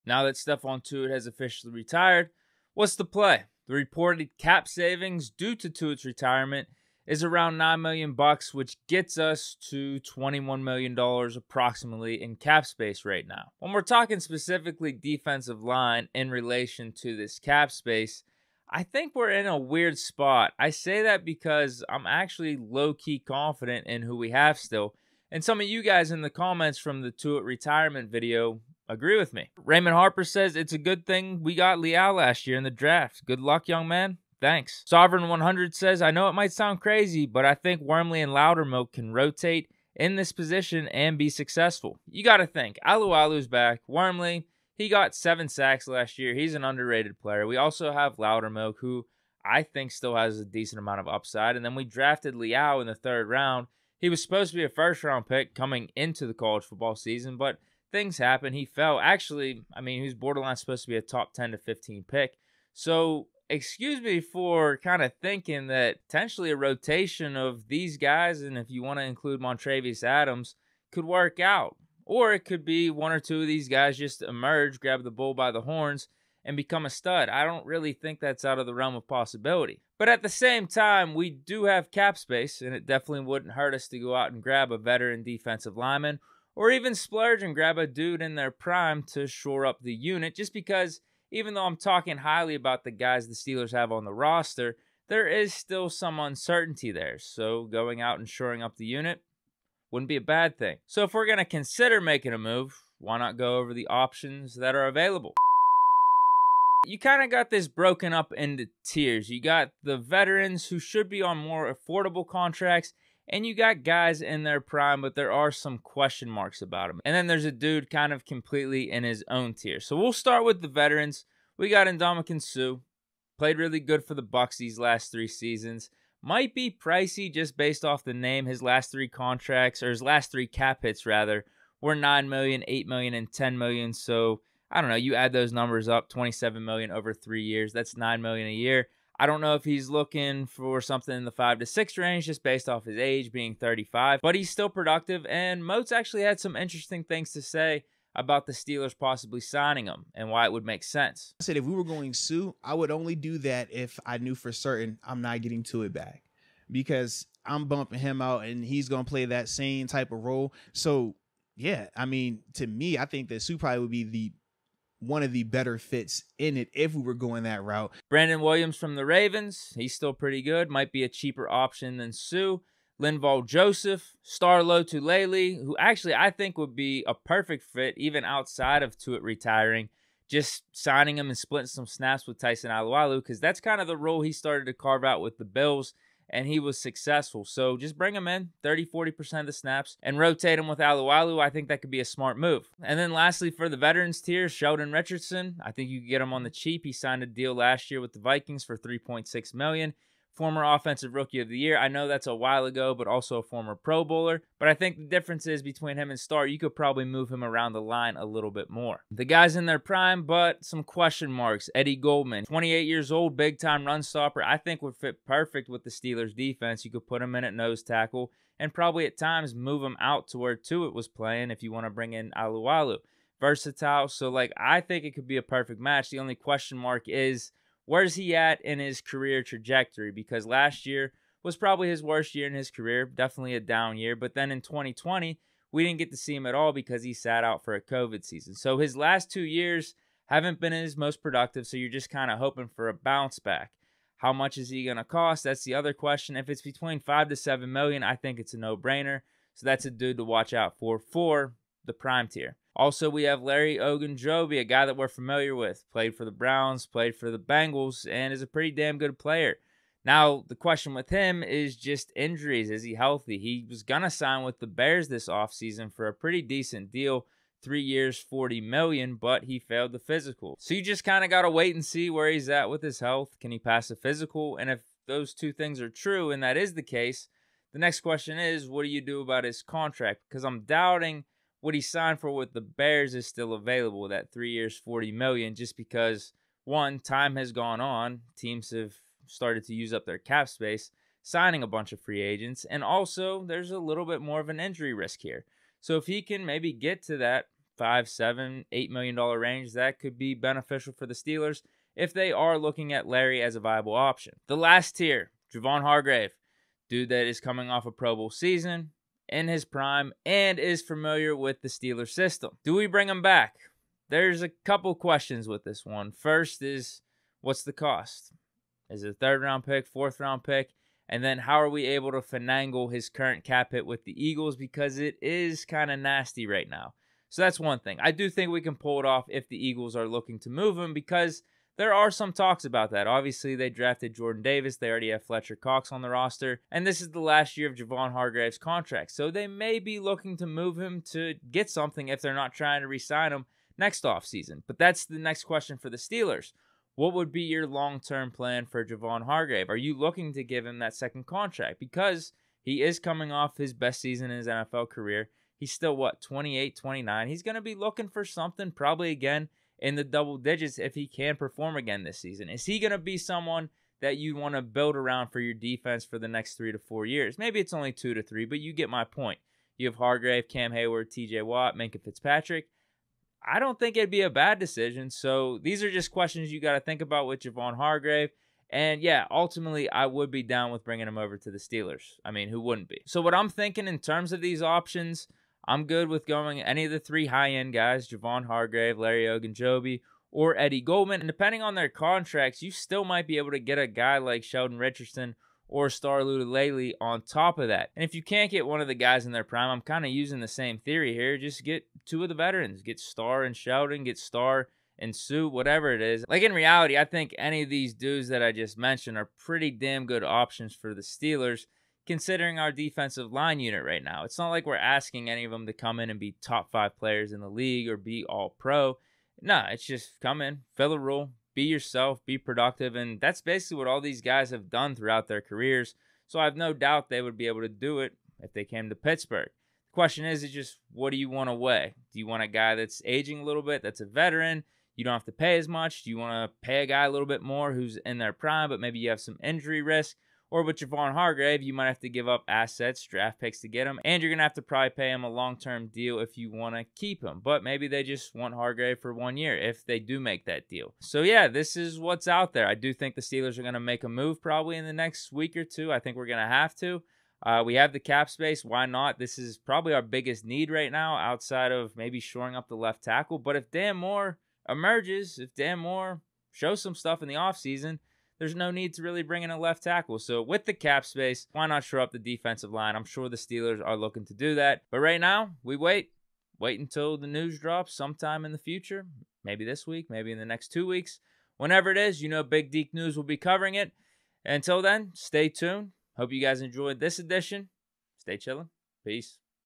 Ranking Defensive Lineman for the Pittsburgh Steelers to add after Stephon Tuitt's retirement
Ranking the available Defensive Linemen in the NFL for the Pittsburgh Steelers to add after Stephon Tuitt decided to retire. Follow on twitch

Pittsburgh Steelers free agents defensive lineman, Pittsburgh Steelers Javon Hargrave, Pittsburgh Steelers Ndamukong Suh, Pittsburgh Steelers Stephon Tuitt